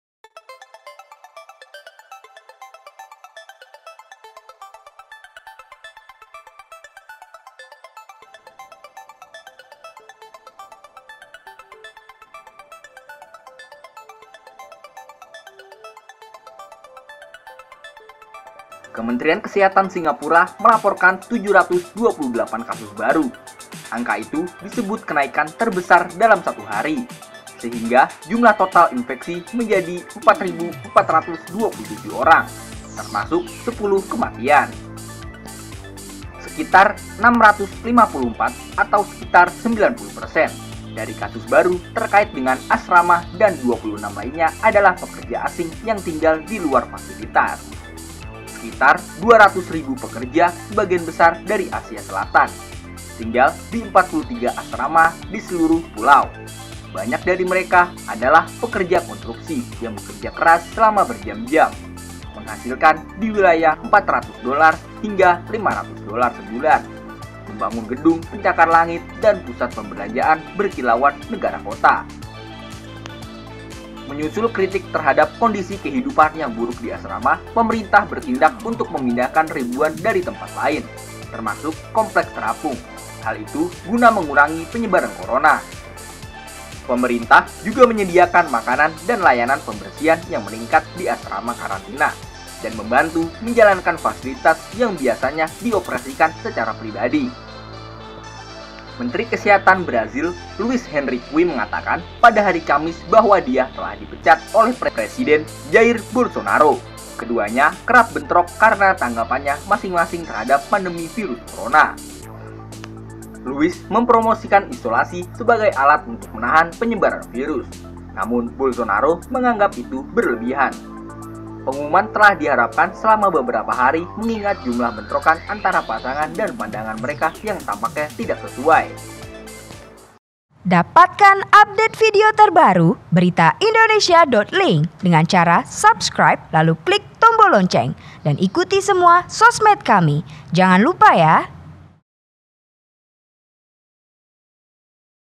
Singapura kenaikan terbesar, Presiden Brasil dan Menkes Berbeda
Singapura
Menkes Brasil

Kementerian Kesehatan Singapura melaporkan 728 informasi baru,disebut kenaikan terbesar dalam satu hari. dan Menteri Kesehatan Brasil Luiz Henrique Mandetta dipecat  oleh Presiden Jair Bolsonaro